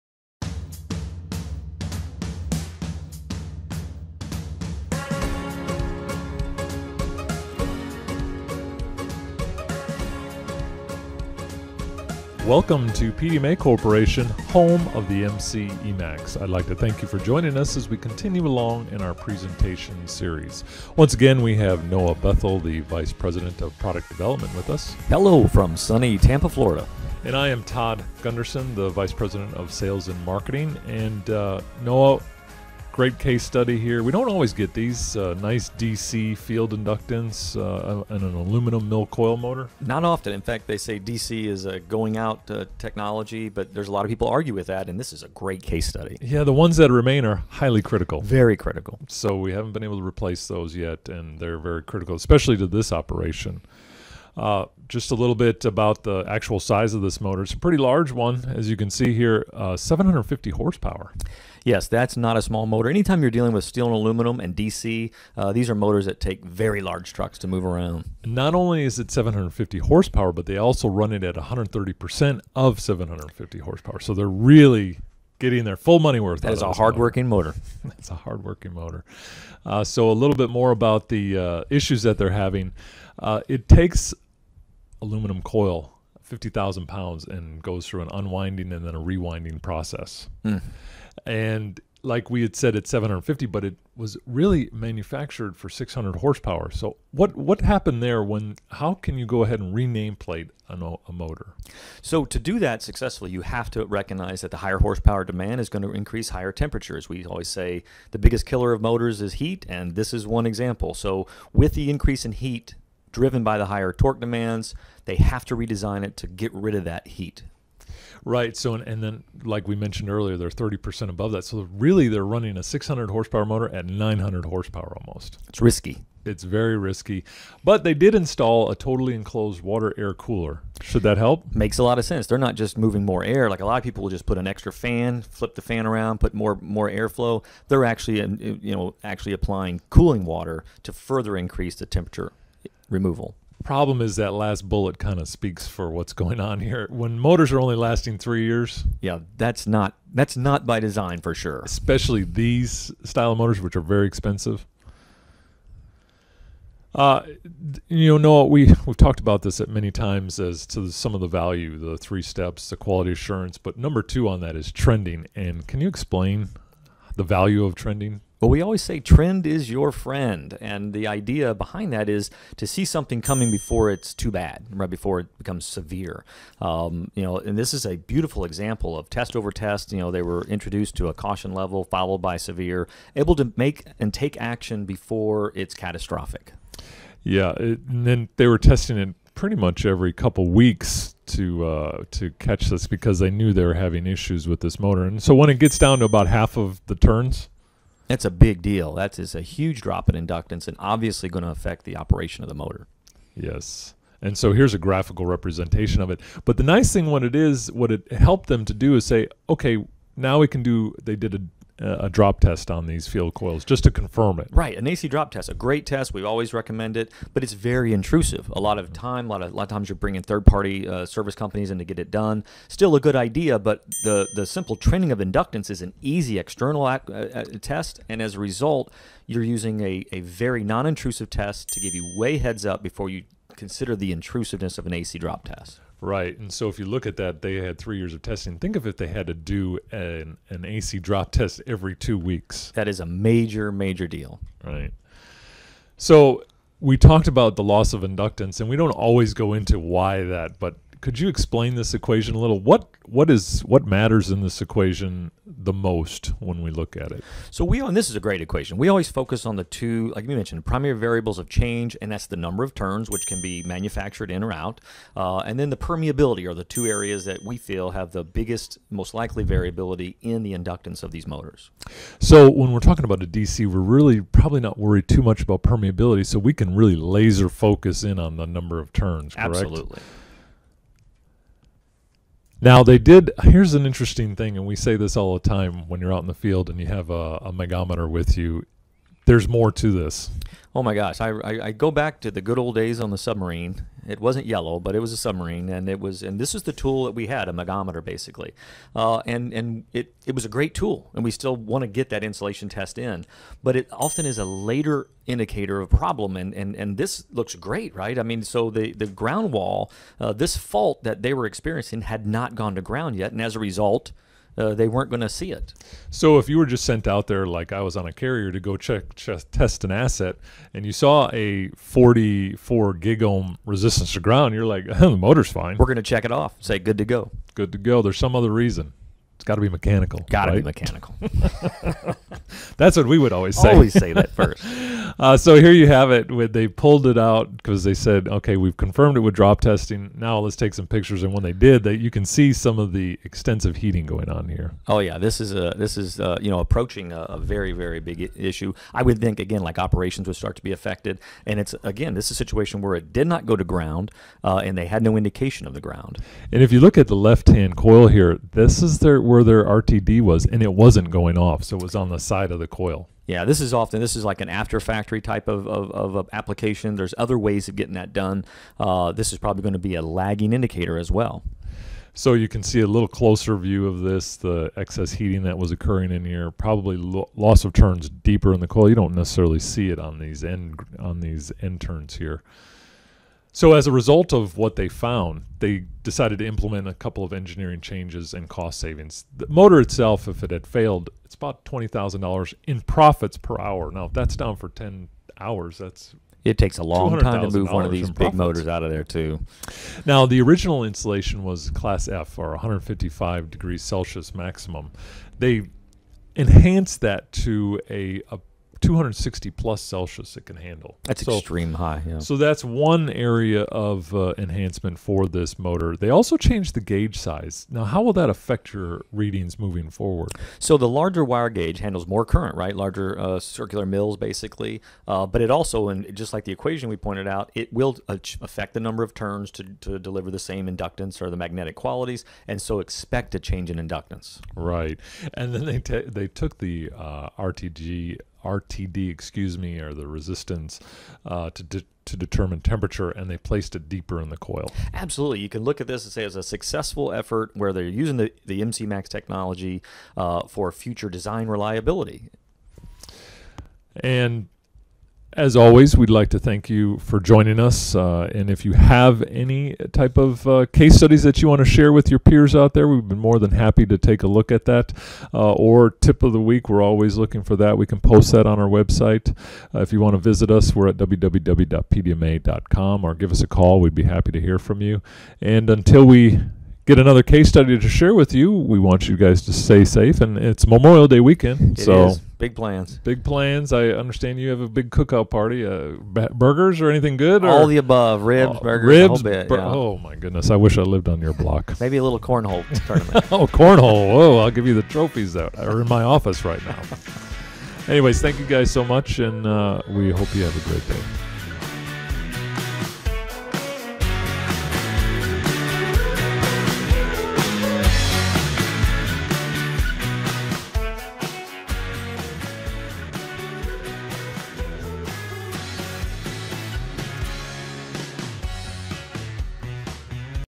Trending DC Field Inductance in An Aluminum Mill Coil Motor
Join Noah Bethel and Todd Gunderson as they discuss the importance of trending DC field inductance measurements for an aluminum coil rewind and unwind motor.